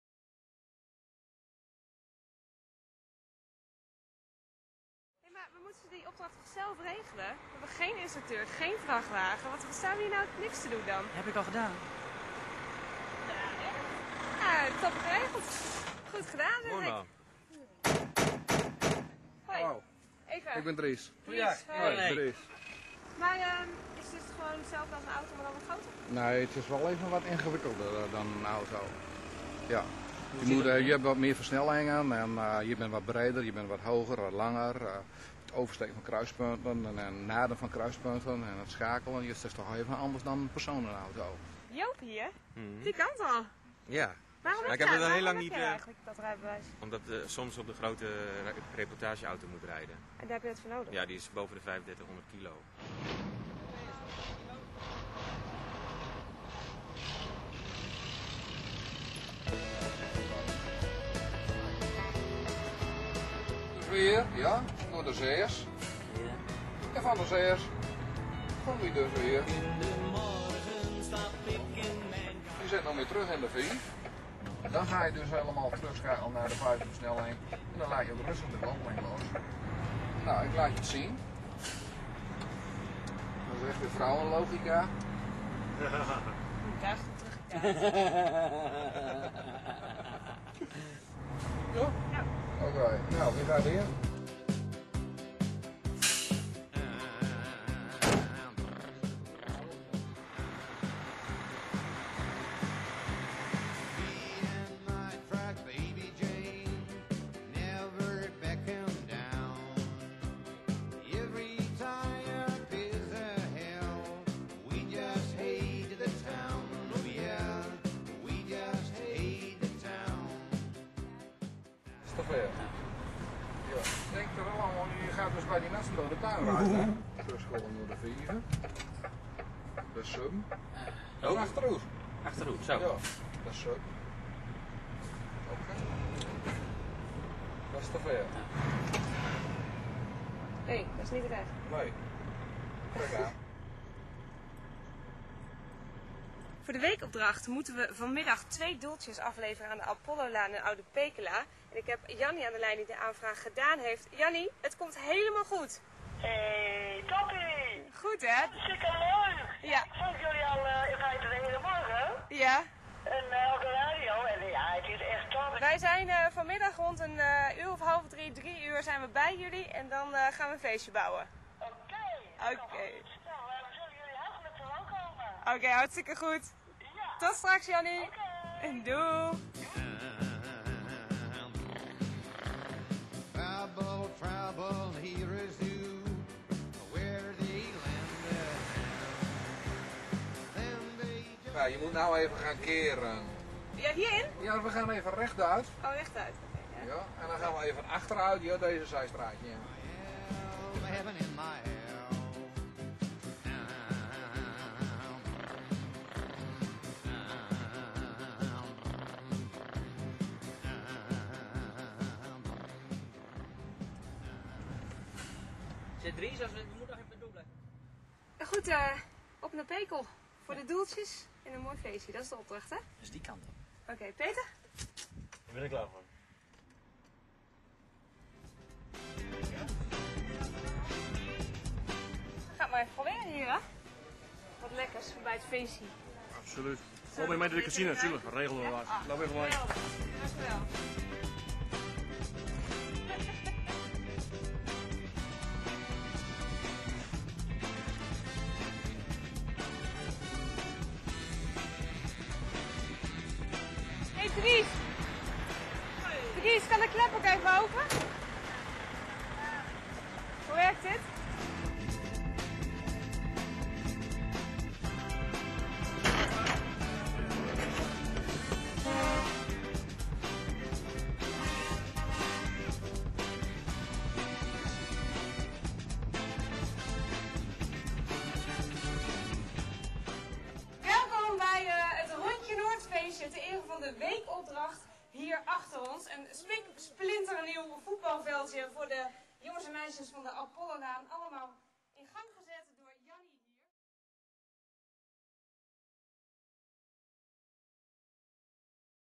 Verkeersschool Schuringa in RTVNoord
Dries Mejeur helpt het team van RTVnoord met de weekopdracht van Winschoten naar Oude Pekela